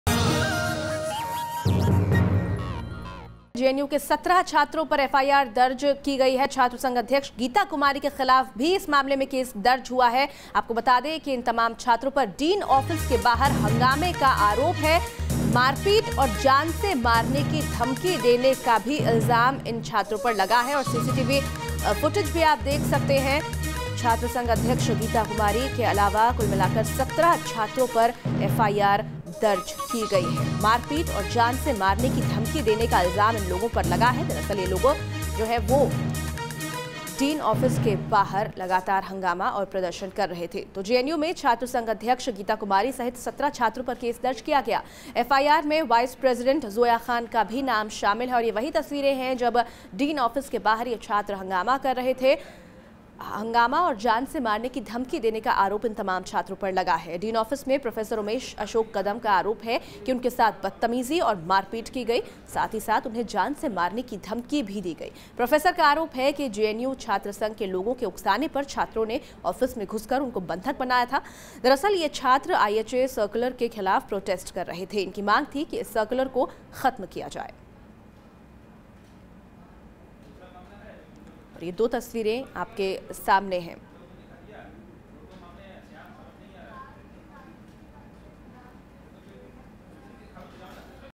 JNU dean files FIR against 17 students for allegedly manhandling him in his office
Umesh Ashok Kadam, the dean of students at Delhi’s Jawaharlal Nehru University (JNU) filed an FIR against 17 students alleging that they assaulted and manhandled him in his office on March 12 over the institute’s attendance rules and hostel fees. Reports said Kadam lodged the complaint four days later, on March 16. The Jawaharlal Nehru University Students’ Union (JNUSU), however, alleged that the dean’s action against them was in response to the sexual harassment complaints against Professor Atul Kumar Johri.